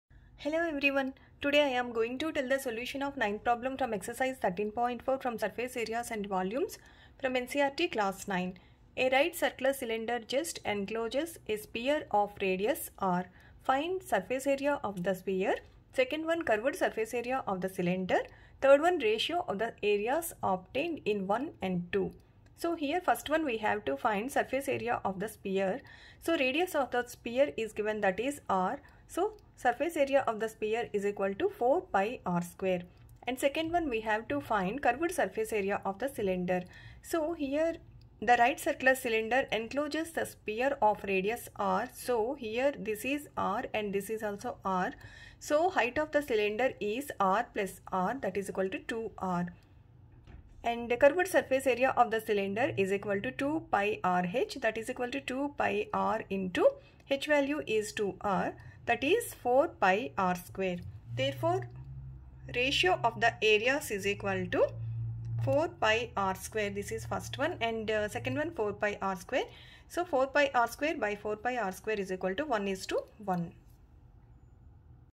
Q9-Ex13.4-Ncert- A right circular cylinder just encloses a sphere of radius r,surface area of the..
Exercise13.4-Ncert- Surface areas and Volumes-Class9
In this video we will learn about the solution of the problem given below.
9. A right circular cylinder just encloses a sphere of
radius r (see Fig. ). Find
(i) surface area of the sphere,
(ii) curved surface area of the cylinder,
(iii) ratio of the areas obtained in (i) and (ii).